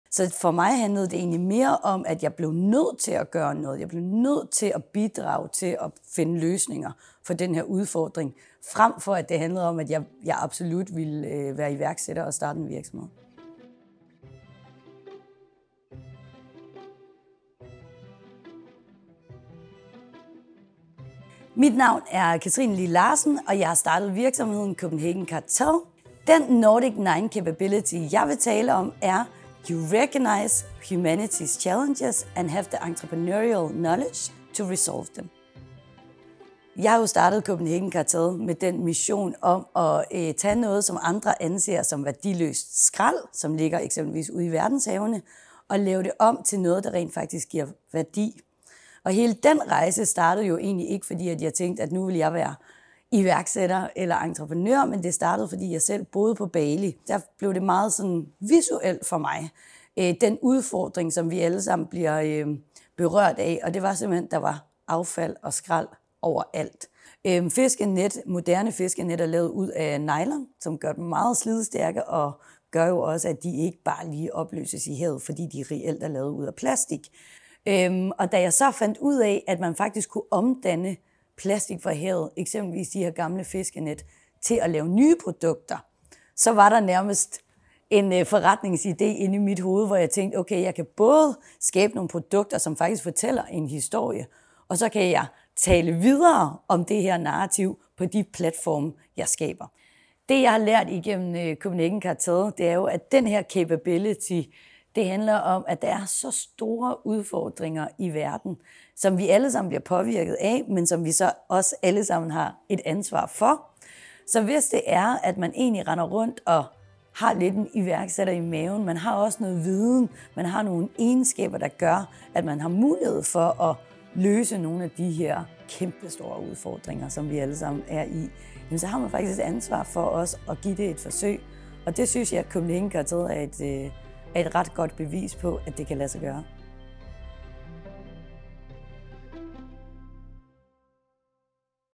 Nordic Nine No 3 - Katrine Lee Larsen
Hear the reflections of Founder and CEO of Copenhagen Cartel, Katrine Lee Larsen, on Nordic Nine No 3: You recognise humanity's challenges and have the entrepreneurial knowledge to help resolve them.